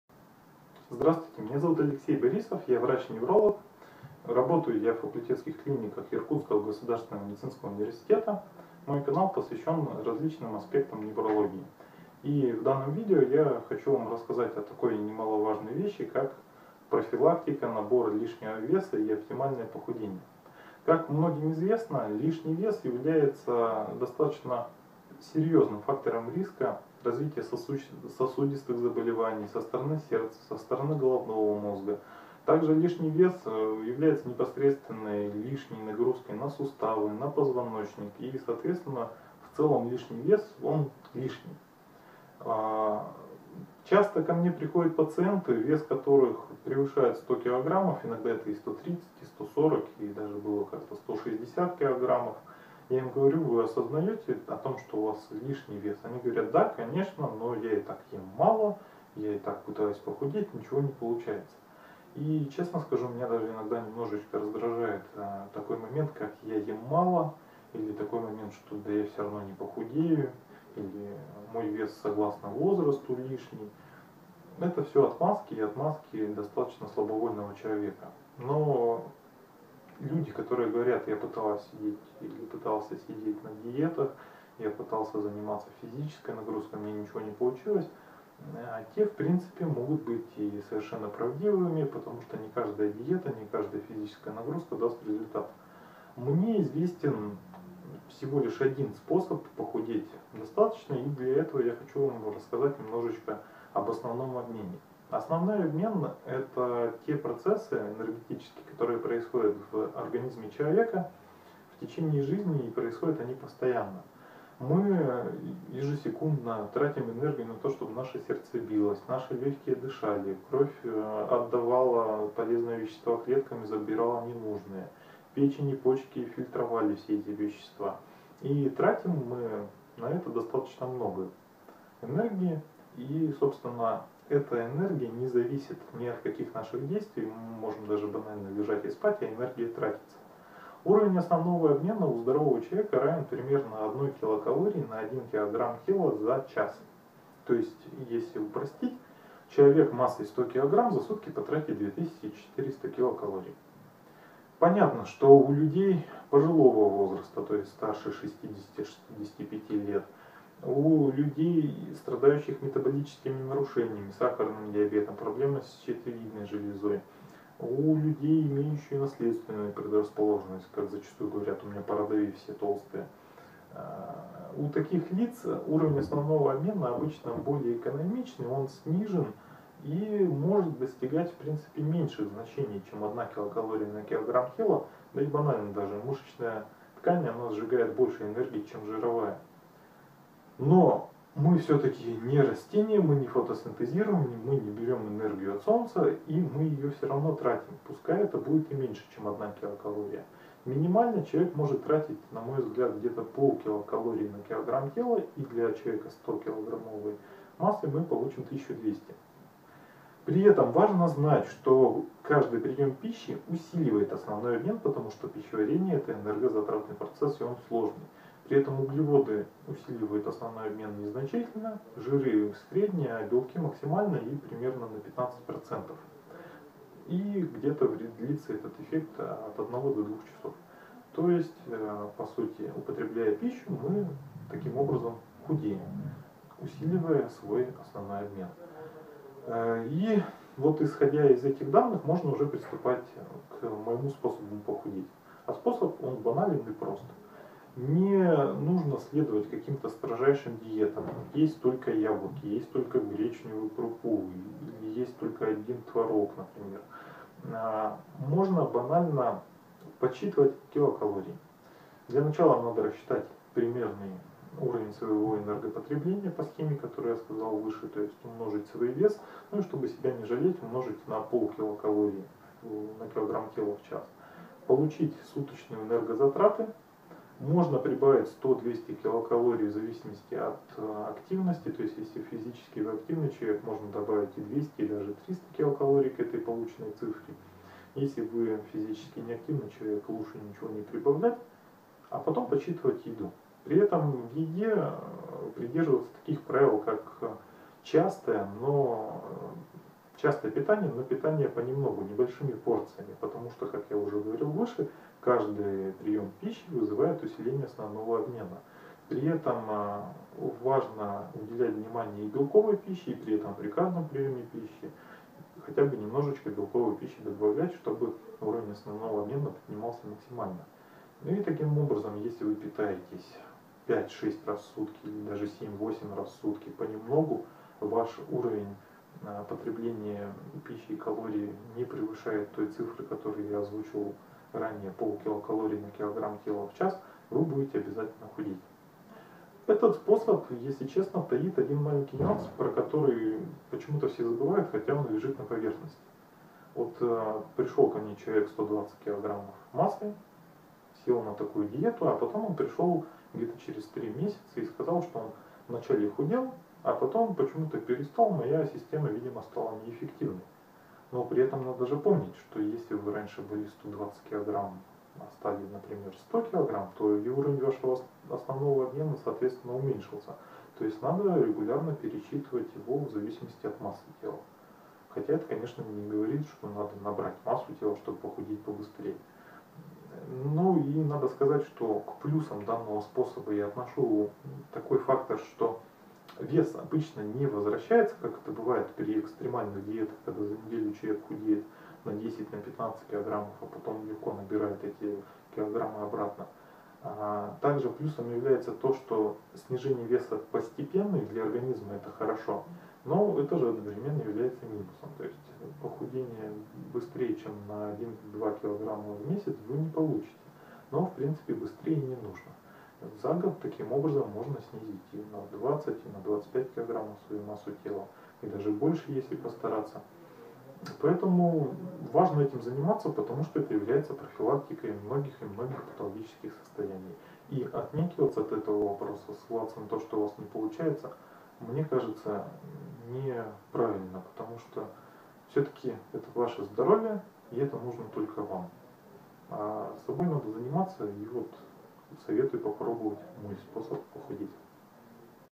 Профилактика сосудистых заболеваний или как сбросить лишний вес?
В видео описан оптимальный и эффективный способ сбросить лишний вес, что способствует профилактике многих сосудистых заболеваний. Больше информации, а также формулы расчетов на